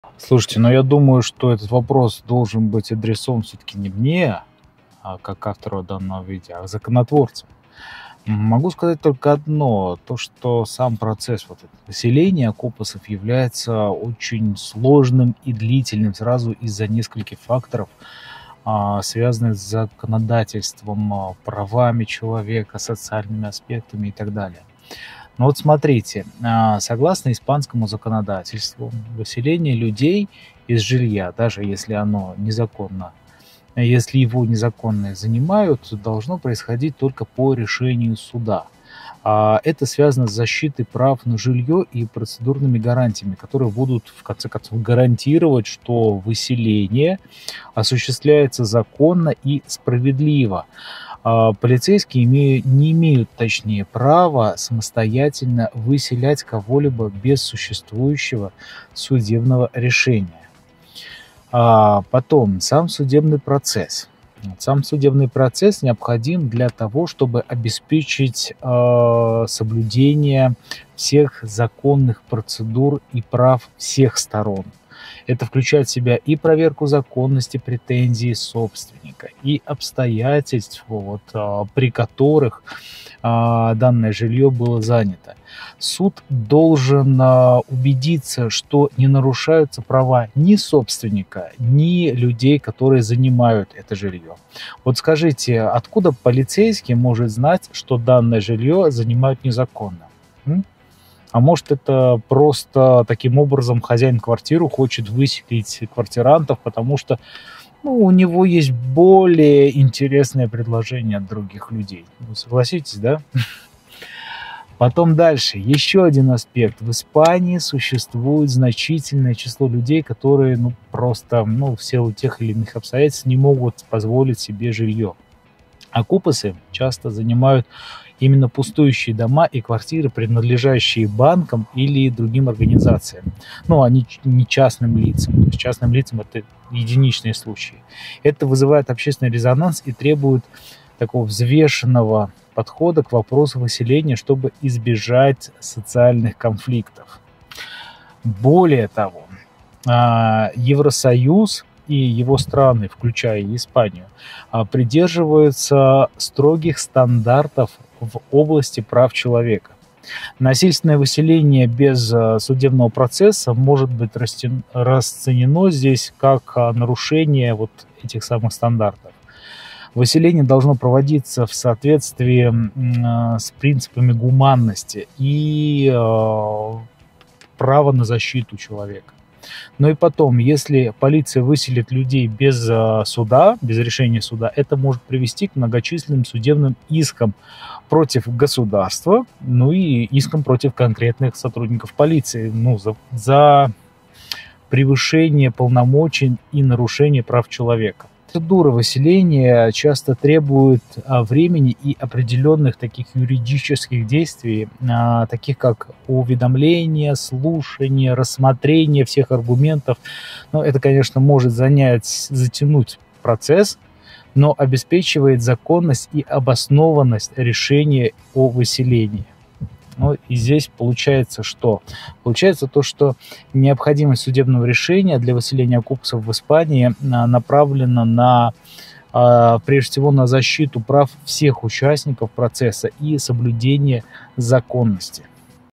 Вот почему окупаса выселять долго и трудно🤷‍♂️
В Испании окупосами называют людей, которые незаконно занимают жильё. Процесс выселения окупосов является сложным и длительным из-за нескольких факторов, связанных с законодательством, правами человека и социальными аспектами.

1. **Законодательные ограничения**: Согласно испанскому законодательству, выселение людей из жилья, даже если они его незаконно занимают, должно происходить по решению суда. Это связано с защитой права на жилье и процедурными гарантиями, которые обеспечивают, что выселение осуществляется законно и справедливо. Полицейские не имеют права самостоятельно выселять кого-либо без соответствующего судебного решения.

2. **Процессуальные гарантии**: Судебный процесс необходим для того, чтобы обеспечить соблюдение всех законных процедур и прав всех сторон. Это включает в себя проверку законности претензий собственника и обстоятельств, при которых жилье было занято. Суд должен убедиться, что не нарушаются права ни собственника, ни людей, которые занимают жилье.

3. **Социальные факторы**: В Испании существует значительное число людей, которые не могут позволить себе жилье. Окупосы часто занимают пустующие дома и квартиры, принадлежащие банкам или другим организациям, а не частным лицам. Это вызывает общественный резонанс и требует взвешенного подхода к вопросу выселения, чтобы избежать социальных конфликтов.

4. **Защита прав человека**: Евросоюз и его страны, включая Испанию, придерживаются строгих стандартов в области прав человека. Насильственное выселение без судебного процесса может быть расценено как нарушение этих стандартов. Выселение должно проводиться в соответствии с принципами гуманности и права на защиту.

5. **Риск судебных исков**: Если полиция выселит людей без решения суда, это может привести к многочисленным судебным искам против государства и конкретных сотрудников полиции за превышение полномочий и нарушение прав человека.

6. **Сложность процедур**: Процедуры выселения часто требуют времени и определённых юридических действий, таких как уведомления, слушания и рассмотрение всех аргументов. Это может затянуть процесс, но обеспечивает законность и обоснованность решения о выселении.

Таким образом, необходимость судебного решения для выселения окупосов в Испании направлена на защиту прав всех участников процесса и соблюдение законности.